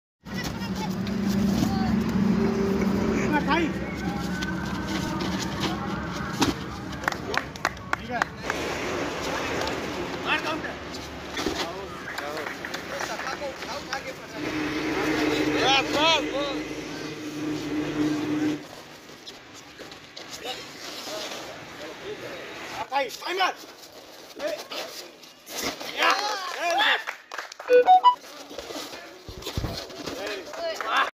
om fight club
Aanand sen # Hariom# P C Pablik: पृथ्वीराज चौहान इंटर कॉलेज बरवा मोतीनगर अयोध्या में जिला स्तरीय# किक बॉक्सिंग प्रतियोगिता संपन्न हुई। जिसमें मुख्य अतिथि पूर्व मंत्री श्री किक बॉक्सिंग प्रतियोगिता का आयोजन ओम फाइट क्लब अयोध्या# के द्वारा किया# गया।om fight club